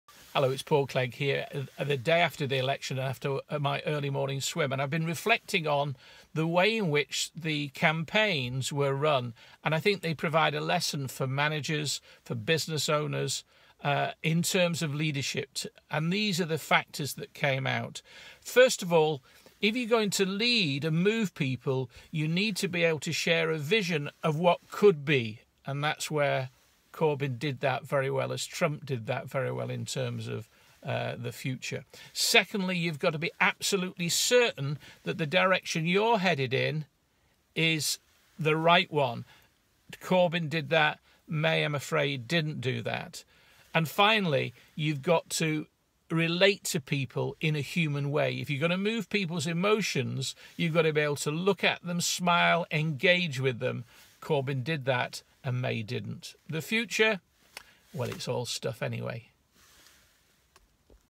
social marketing - 3 Leadership Lesson from Jeremy Corbyn
Put your politics to one side for a moment. We can learn leadership and entrepreneurial lessons from the way Jeremy Corbyn approached the UK election on 8th June.